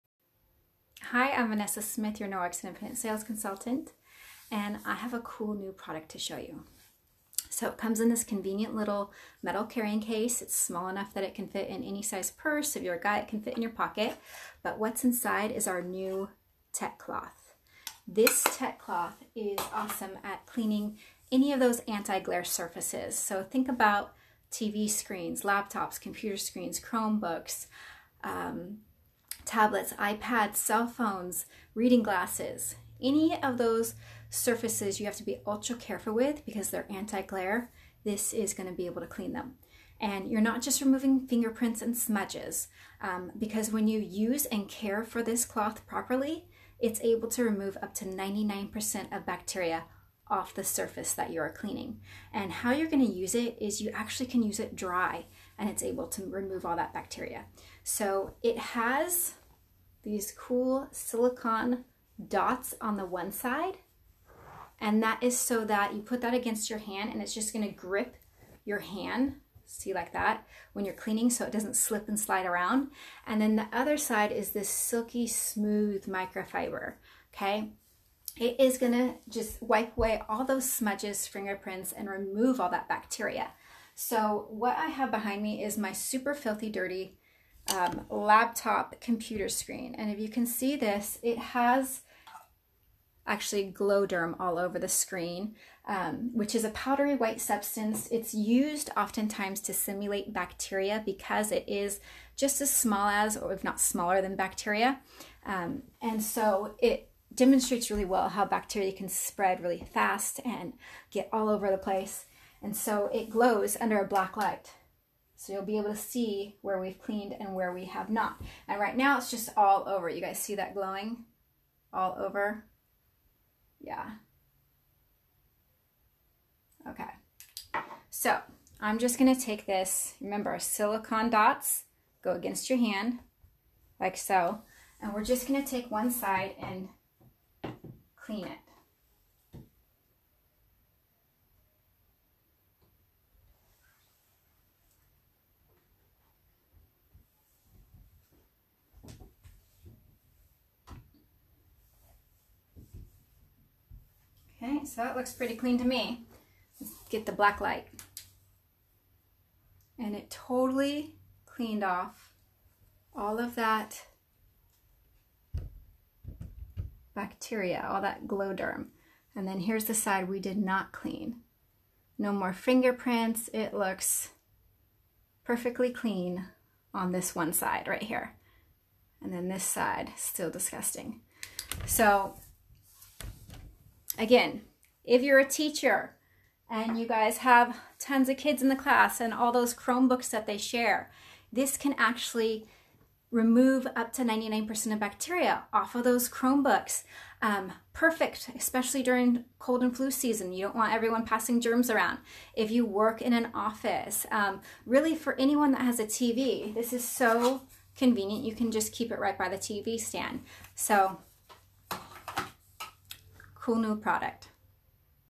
Norwex tech cleaning cloth
Glow derm demo of the norwex tech cleaning cloth showing how it can remove up to 99% of bacteria from the surface.